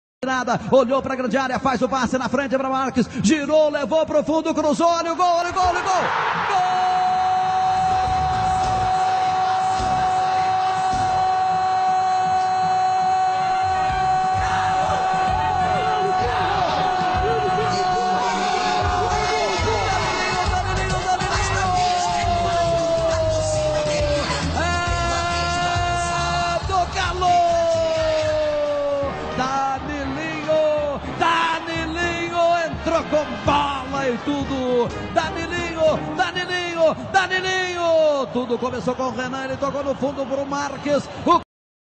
Atlético-MG 1 x 0 Nautico - Copa do Brasil 2008
Pela Copa do Brasil, o Atlético venceu o Nautico Capibaribe.

Gol do Atlético-MG: Danilinho

Narração: Milton Naves
Rádio Itatiaia